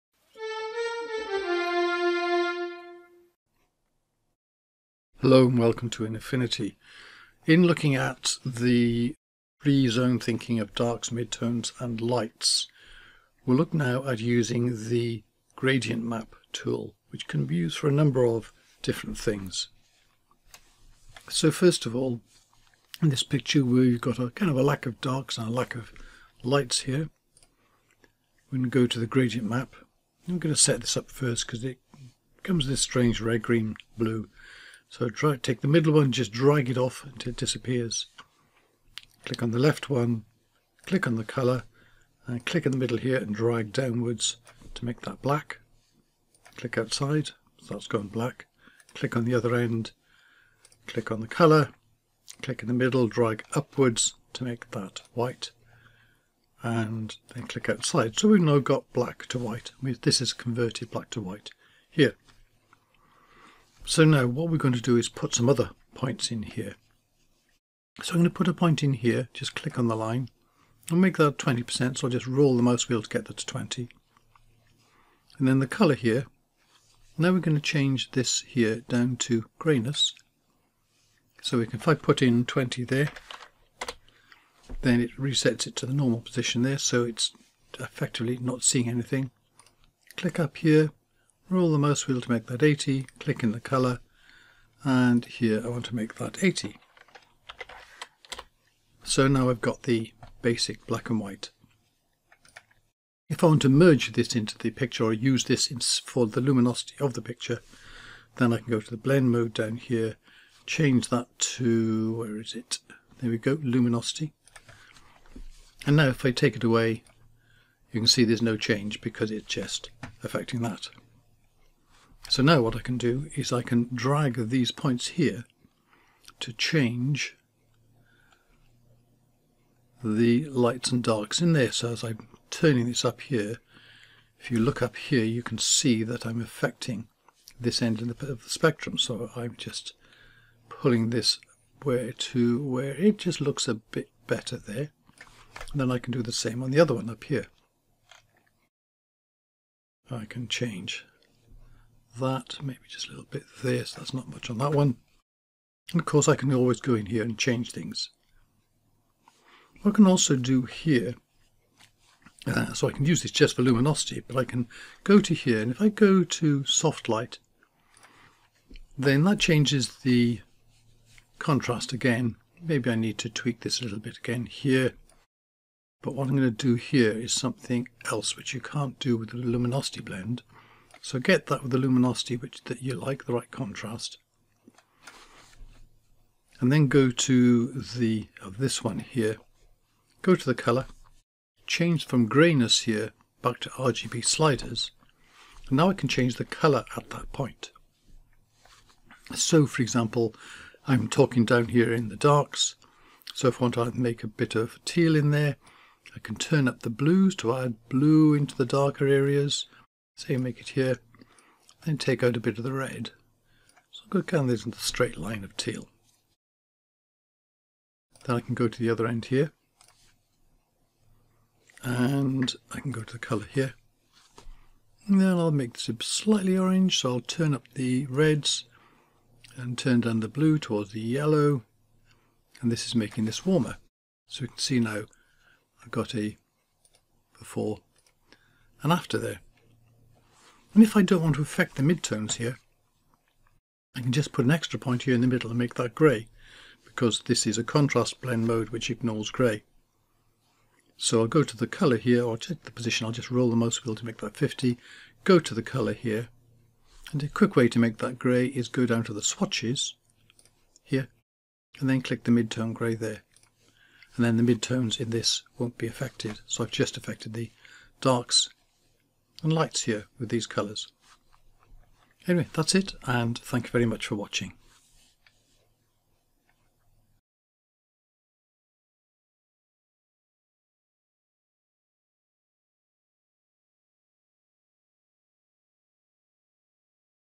Three-Zone Thinking 5: Using a Gradient Map for Tones and Colour
In our series on editing for the three zones of darks, midtones and lights (or shadows, midtones and highlights), here we show how you can use the Gradient Map adjustment to either adjust only luminosity or to change the colour of the zones (or even leave such as midtones unchanged).